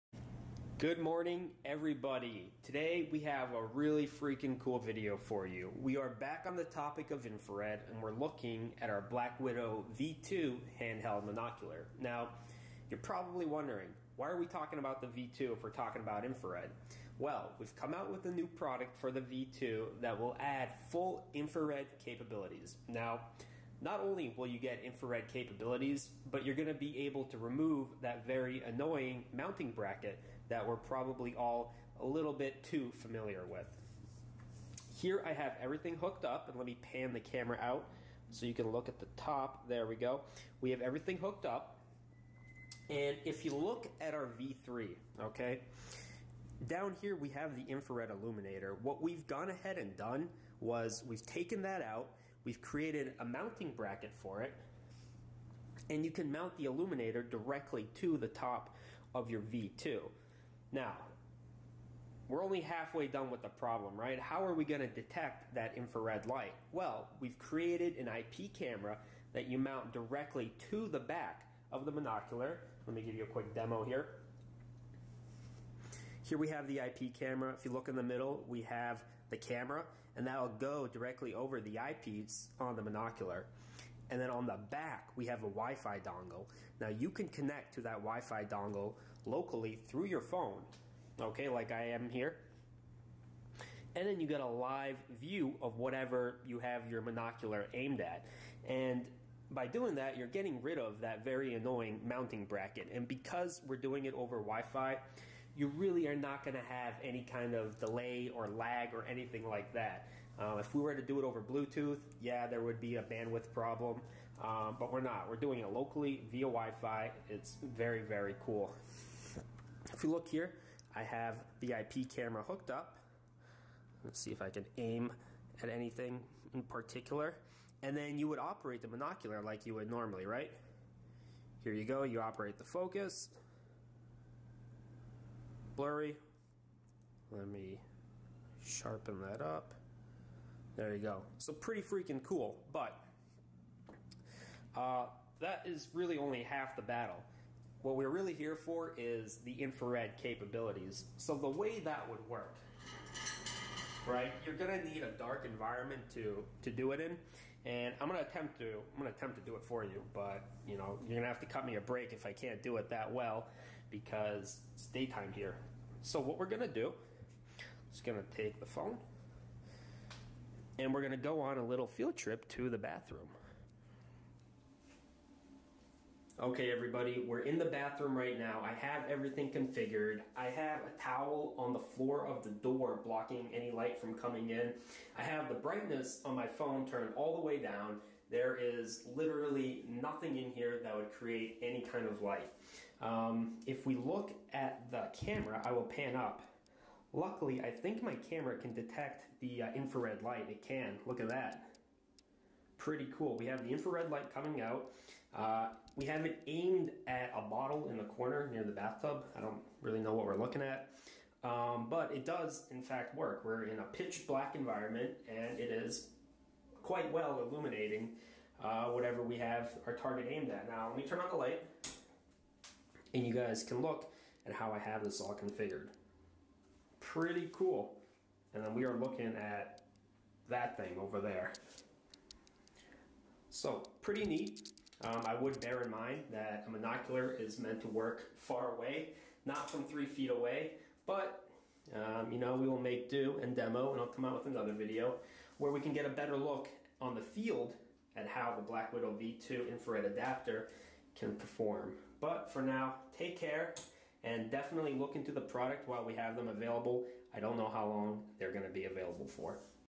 Aviator Pocket Optic V1 Monocular Infrared Demo
We are taking a look at this infrared accessory designed for use with the Black Widow V2. It works through the use of an infrared illuminator and IP camera which can be connected to locally via Wi-Fi.